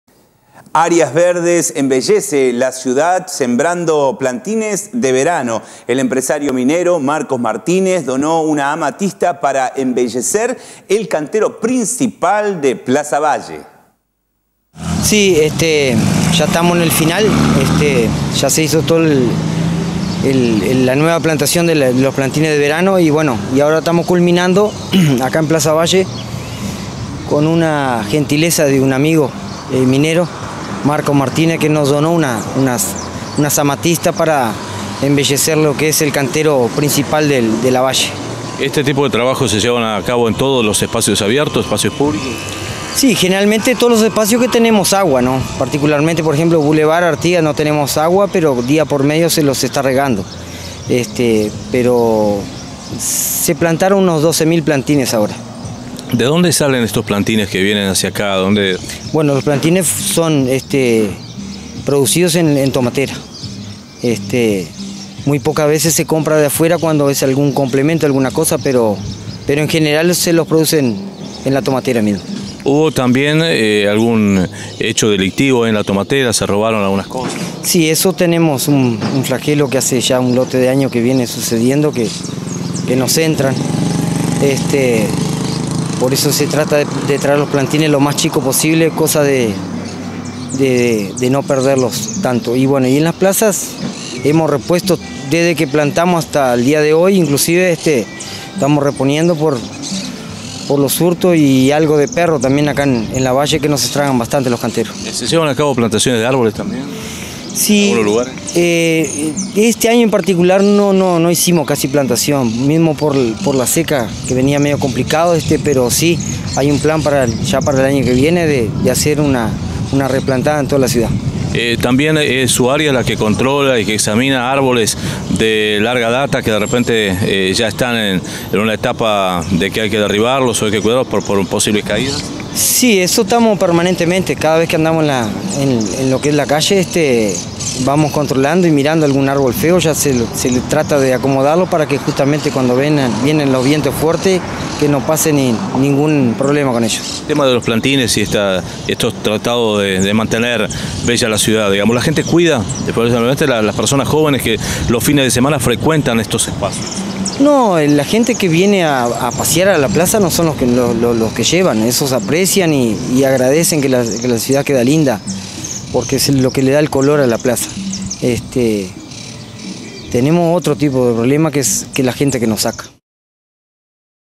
Infotres - 101220 - Ricardo Brazeiro
Áreas Verdes embellece la ciudad sembrando plantines de verano